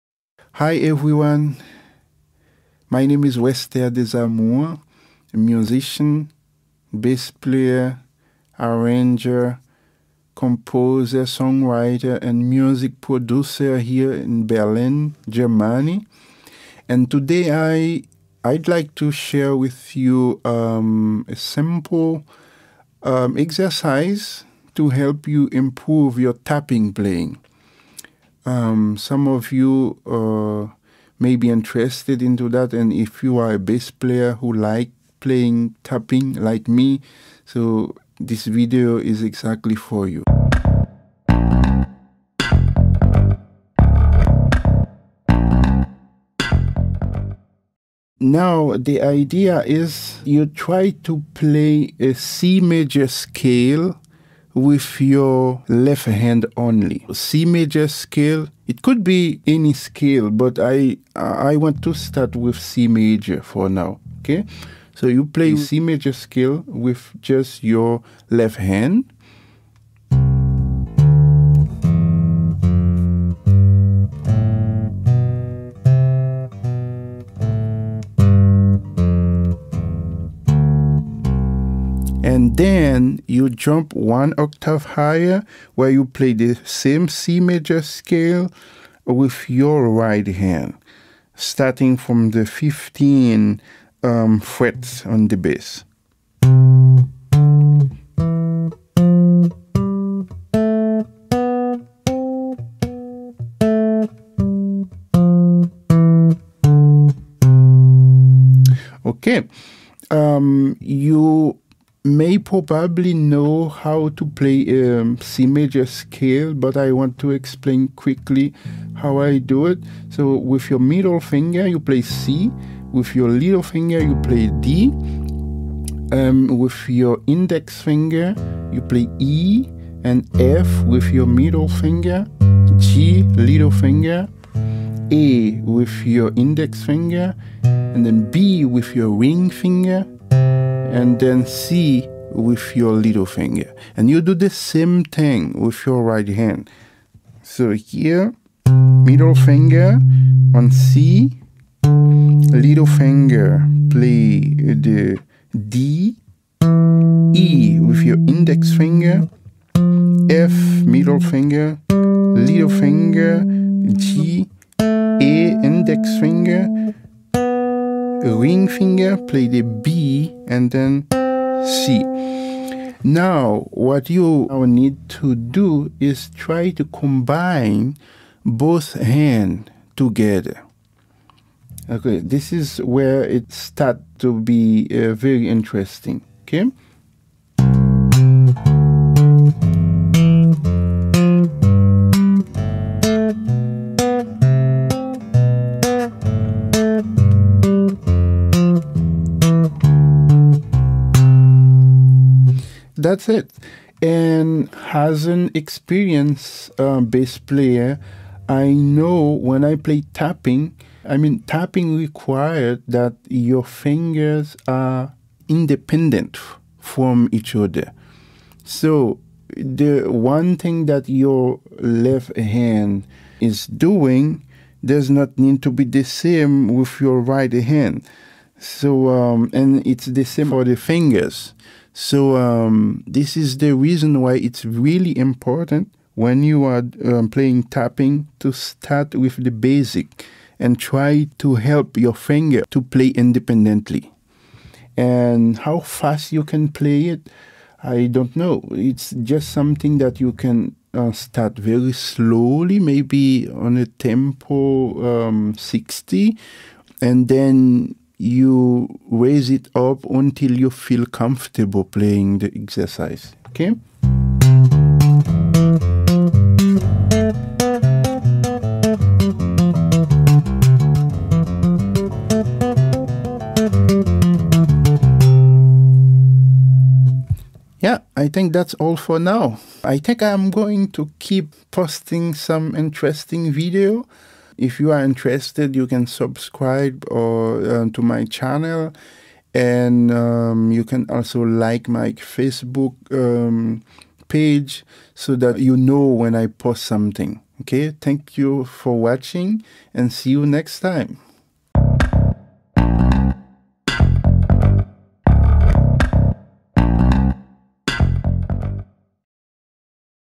TAPPING BASS LESSON 1 (Learn to play the C-Maj scale together with both right and left hands)
Here's a place where you can find out more about what a bass guitar can do.